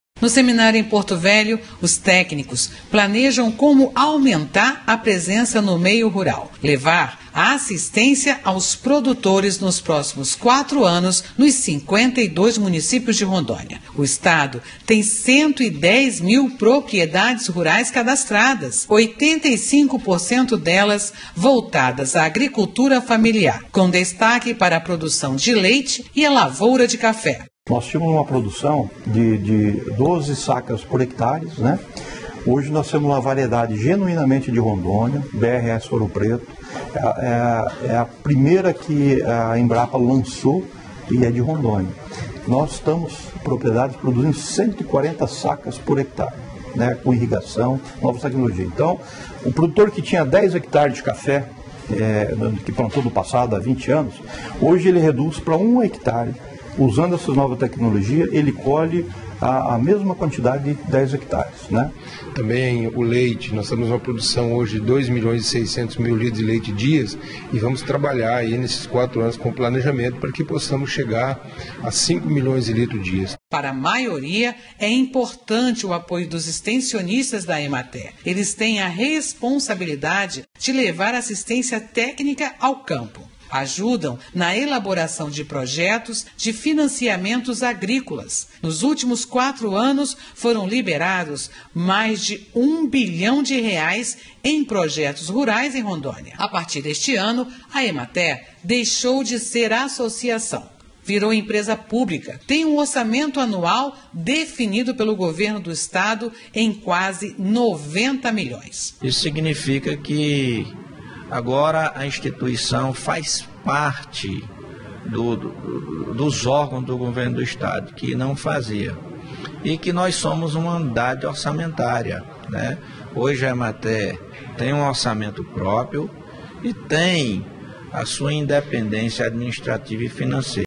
Amazônia News l Emater planeja ações para atender Agricultura Familiar no Estado
Emater planeja ações para atender Agricultura Familiar no Estado